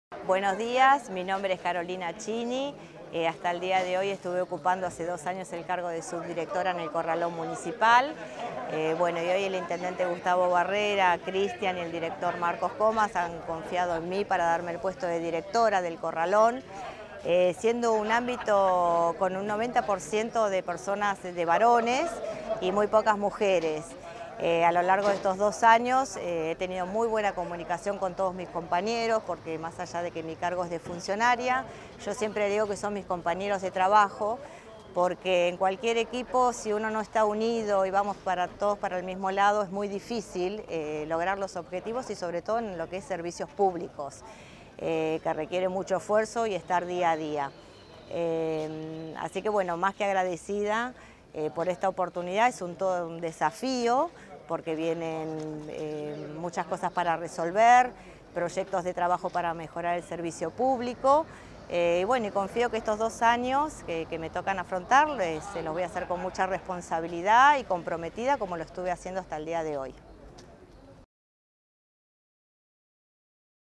Carolina Chinni es la primera mujer a cargo del Corralón Municipal 👷🏻‍♀️
La flamante directora ya desarrollaba funciones en este sector en el que predominan los hombres y destacó el valor de la unión para lograr los objetivos 💪🏼

🙌🏽 Agradeció la oportunidad y adelantó cómo afrontará los desafíos de los próximos dos años.